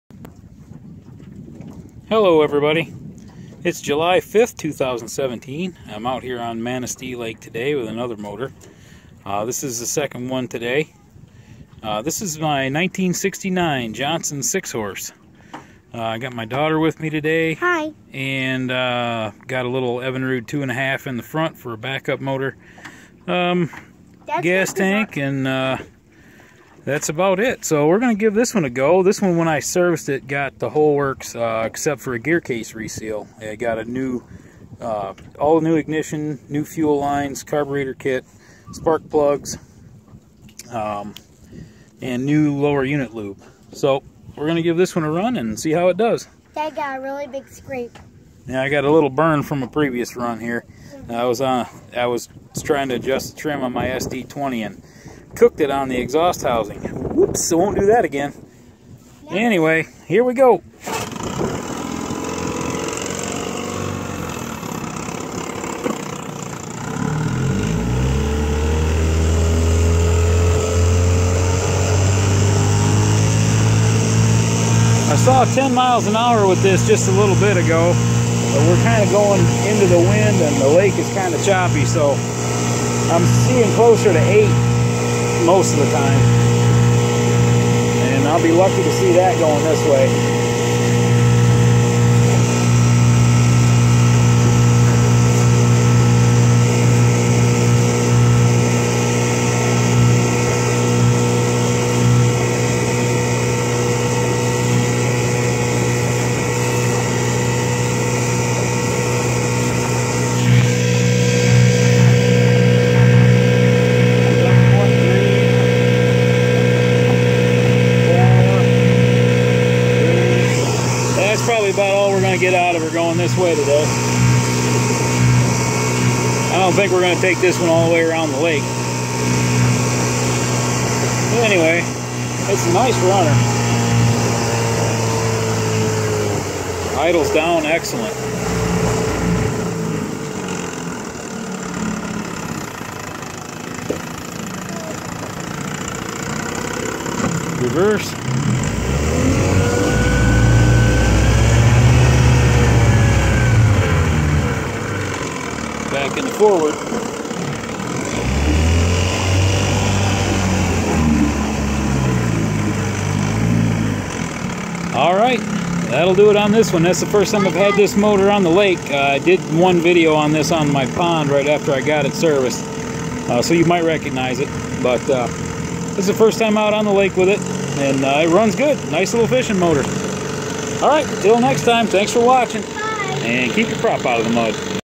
1969 Johnson 6hp Outboard Motor on Manistee Lake
First Lake run with this motor.  It runs great. Lake was a bit choppy and that slowed it down.  I still saw 10.1mph once out of it on this run.